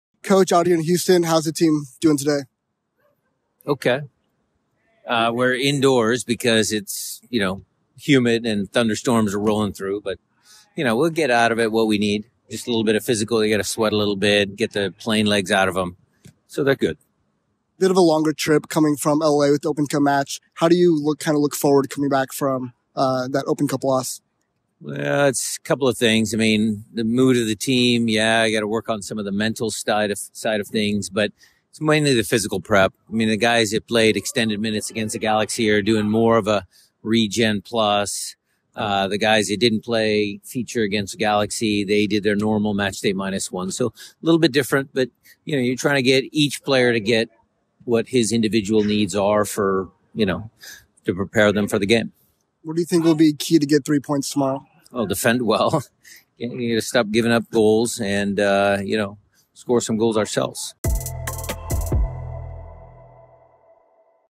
Interview: Brian Schmetzer on preparing to face Houston Dynamo on the road
Seattle Sounders FC Head Coach Brian Schmetzer speaks to media on preparing to face Houston Dynamo on the road.